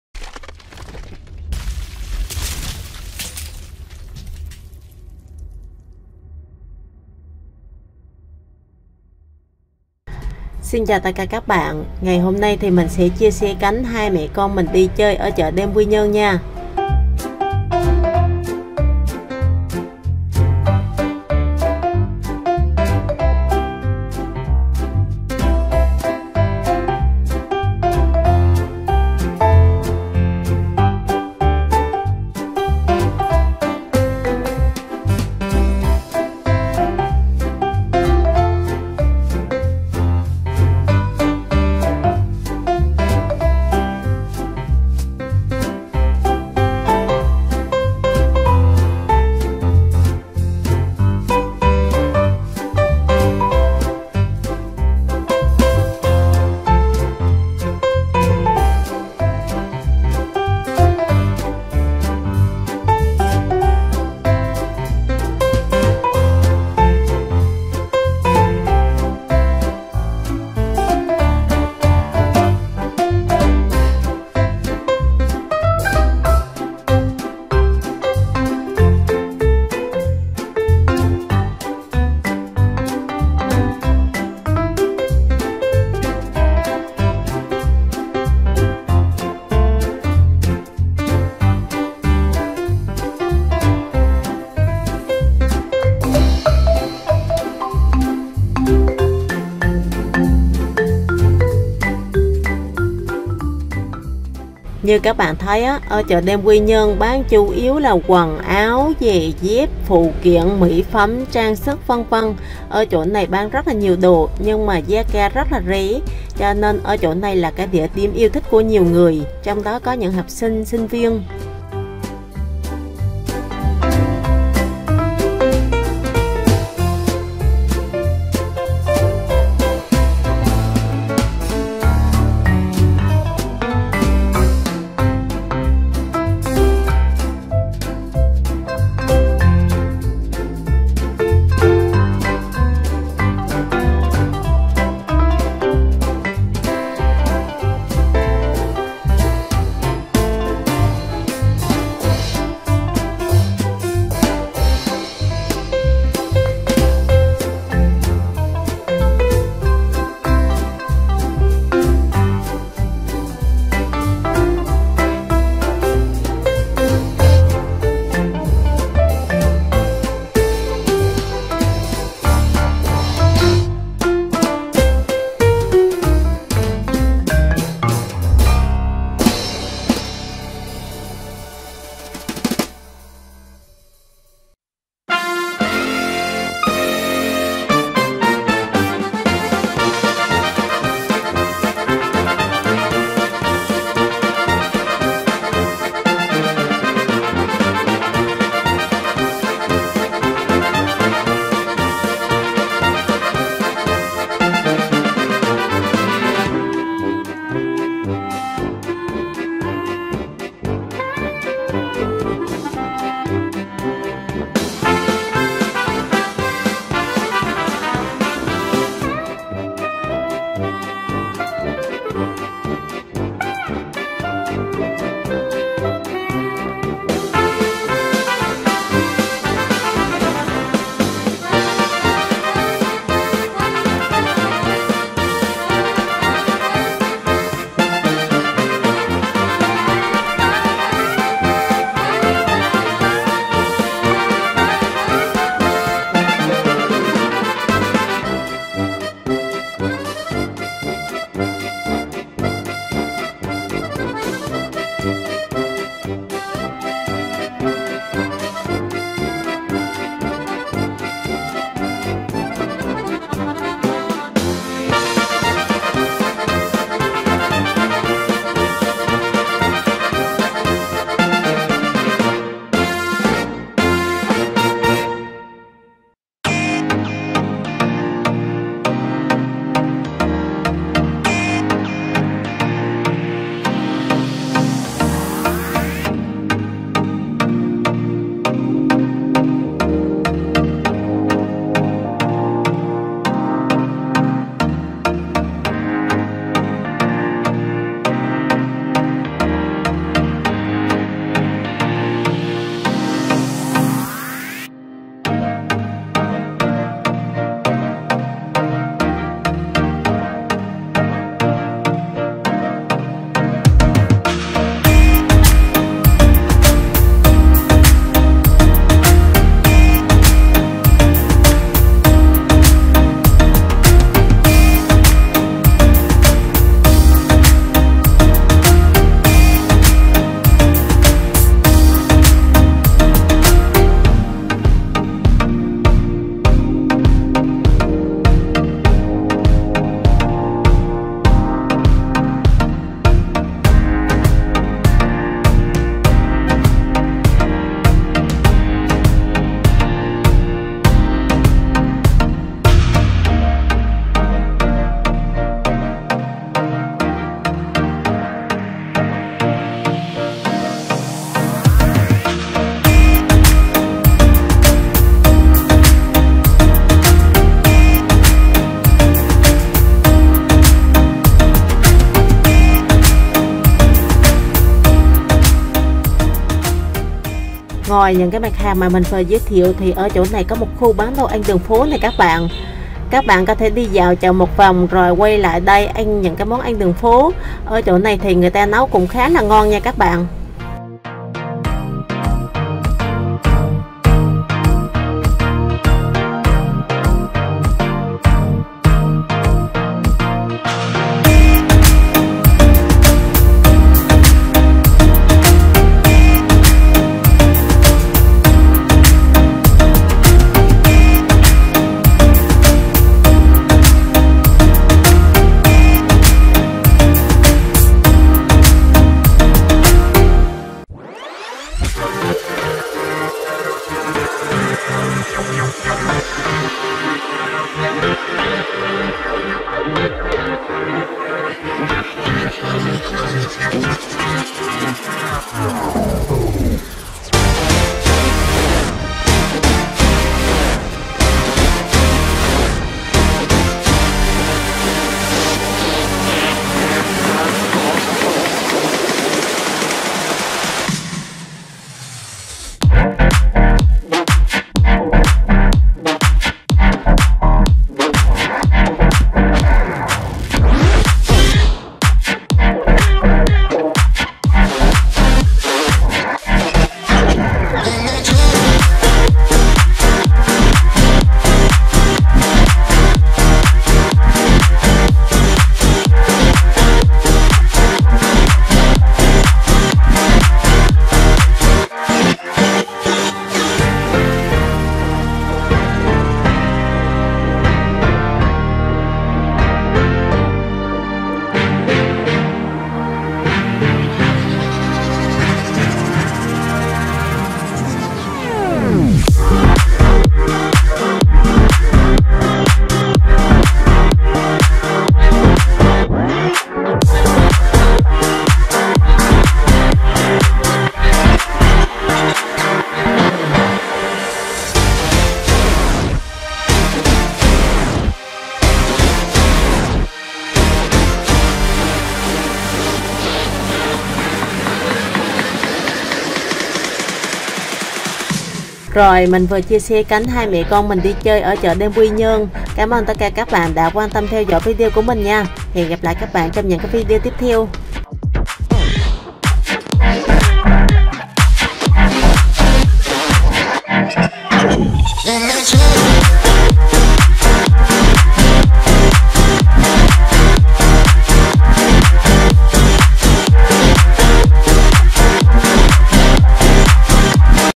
Hai Mẹ Con Dạo Chợ Đêm Quy Nhơn.
Video clip: Video mình chia sẻ lại cảnh hai mẹ con mình đi dạo ở chợ đêm Quy Nhơn.

Các bạn thấy hay thì nhớ nhấn nút [ ĐĂNG KÝ ] kênh để ủng hộ mình nhé..!

Lưu ý: Đẹp Ghê Chưa TV không sở hữu tất cả tư liệu được sử dụng trong video này.